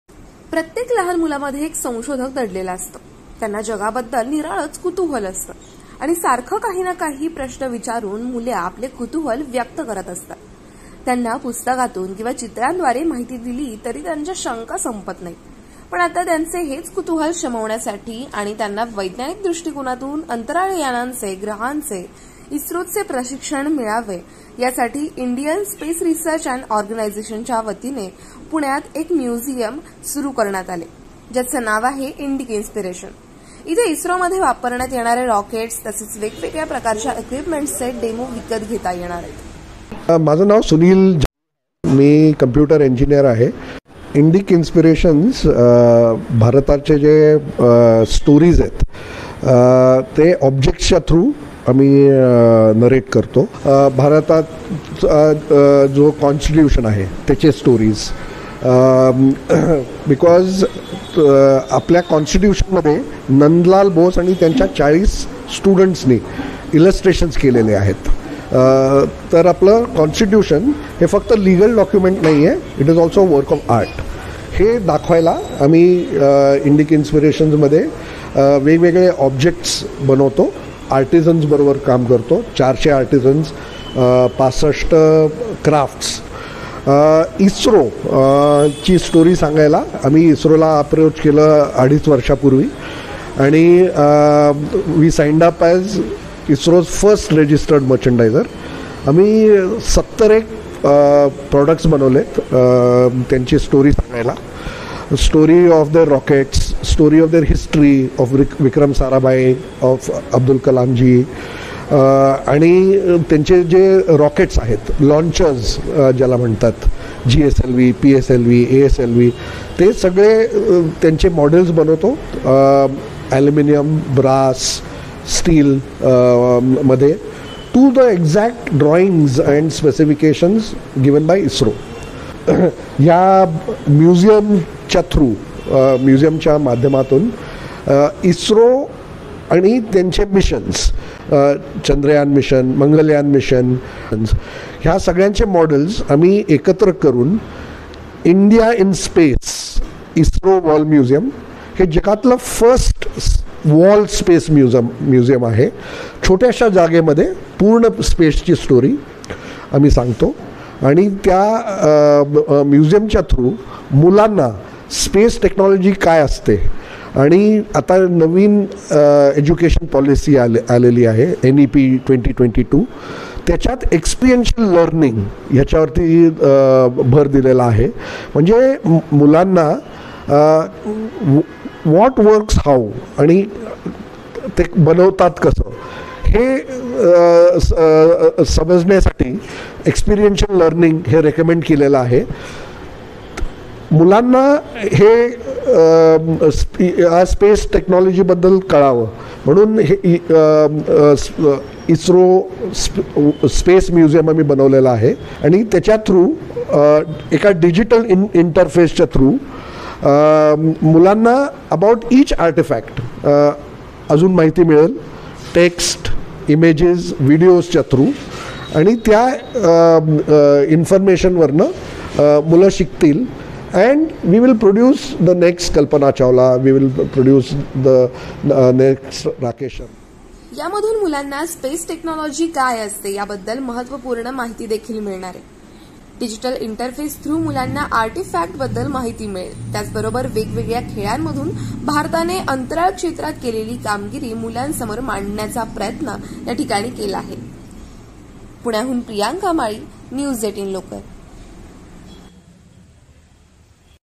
Pune News : बच्चे कंपनीसाठी खुलं झालं इस्रोचं जग, खेळातून मिळणार अंतराळाचा धडा, Video
प्रियांका माळी, प्रतिनिधी

पुणे, 19 जून : प्रत्येक लहान मुलांमध्ये एक संशोधक दडलेला असतो. त्यांना जगाबद्दल कुतुहूल असतं. हे कुतुहल शमवण्यासाठी ते निरनिराळे प्रश्न विचारतात. मुलांना अनेकदा पुस्तकं दिली तरी त्यांचं समाधान होत नाही. त्यांची ही जिज्ञासा पूर्ण करण्यासाठी पुण्यात इस्रोकडून खास म्युझियम सुरू करण्यात आलंय.
कसं आहे म्युझियम?
जगातील प्रमुख अंतराळ संस्था असलेल्या इंडियन स्पेस रिसर्च अँड ऑर्गनायझेशन म्हणजेच इस्रोच्या वतीनं हे म्युझिअम सुरू करण्यात आलंय. 'इंडिक इन्सिपेरशन' असं या म्युझिअमचं नाव आहे. या माध्यमातून मुलांना इस्रोचं जग खुलं झालं आहे.

'या म्युझियममध्ये इस्रोमध्ये वापरण्यात येणारे रॉकेट्स तसंच वेगवेगळ्या उपकरणांचे डेमो विकत घेता येतील. त्याचबरोबर इस्रोच्या रोमहर्षक इतिहासाची माहिती देखील मुलांना या म्युझियममधून मिळणार आहे.
'इंडिक इन्सिरेशनमध्ये वेगवेगळे ऑब्जेक्ट्स बनवण्यात आले आहेत. यामध्ये 400 कार्डेजन्स आणि 70 प्रॉडक्टचा समावेश आहे. या प्रत्येकाची एक खास गोष्ट आहे. स्टोरी ऑफ द रॉकेट, स्टोरी ऑफ अब्दुल कलाम, त्यांचे जे रॉकेट्स याचे मॉडेल्स इथं बनवण्यात आलेत. ॲल्युमिनियम, ब्रास स्टीलमध्ये यानांचे तसंच स्पेस क्राफ्टचे नेमके ड्रॉईंग तयार करण्यात आलेत. यामधून मुलांना स्पेस टेक्नॉलजी म्हणजे काय हे समजण्यात मदत होईल,' अशी माहिती येथील आयोजक सुनील जक्कार यांनी दिली.
9 वीपासून टीव्ही पाहणं केलं बंद, डोंबिवलीकर अविनाशचा CET नंतर JEE मध्येही डब्बल धमाका, Video
'यामधील डिजिटल इंटरफेसच्या माध्यमातून आर्टिफॅक्टबद्दलही मुलांना माहिती मिळणार आहे. भारतानं अंतराळ क्षेत्रात केलेली कामगिरी वेगवेगळ्या खेळांच्या माध्यमातून मुलांसमोर मांडण्यात आली आहे.
कुठं आहे म्युझियम?
मुंशी चेंबर्स, पासपोर्ट ऑफिस जवळ कोरेगाव पार्क एनेक्स, घोरपडी, मुंडवा रोड, पुणे, 411036,